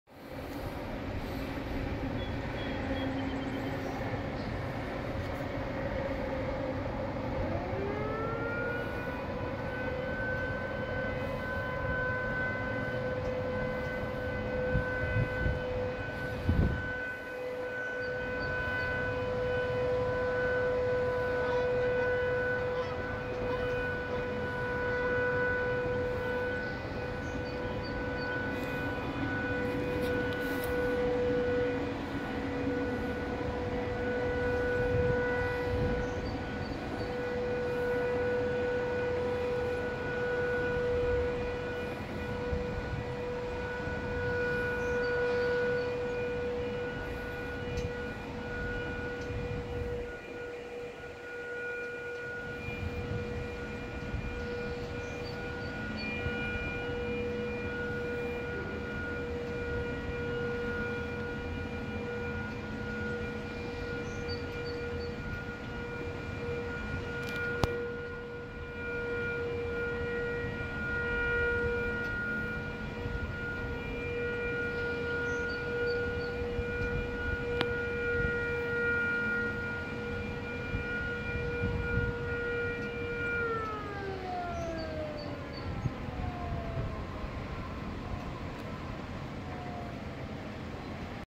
Lacon, IL Siren Ambience
First Tuesday test of Lacon's Model 2
please dont tag matthew english